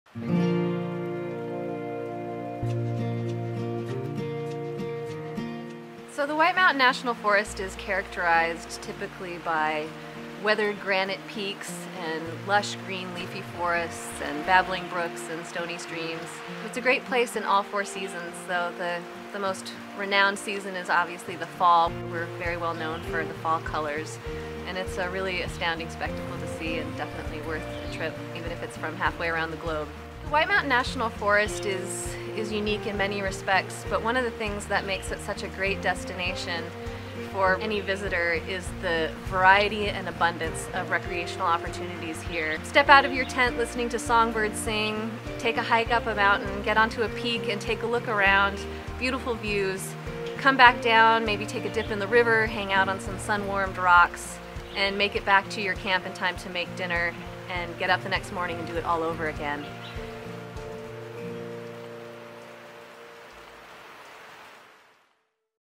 Parques: Floresta Nacional de White Mountain
A Floresta Nacional de White Mountain, em New Hampshire, é um destino fantástico para apreciar a vida ao ar livre, repleto de atividades como trilhas, acampamentos e muito mais.